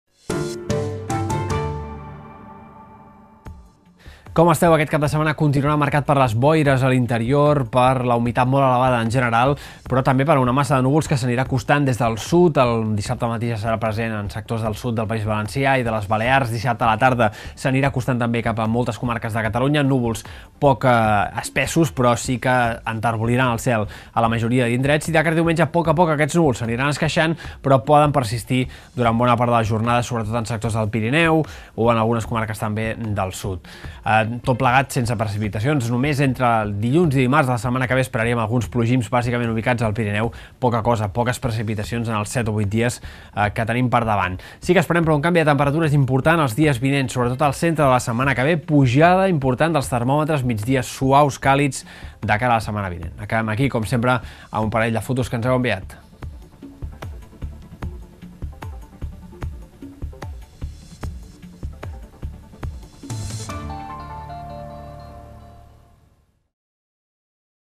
La méteo en 1 minut: cap de setmana amb boires i més núvols
Entre dissabte a la tarda una massa de núvols enterbolirà el cel a moltes comarques sense deixar pluges. La boira persistirà bona part del cap de setmana a Ponent. A hi trobareu la previsió per al vostre municipi.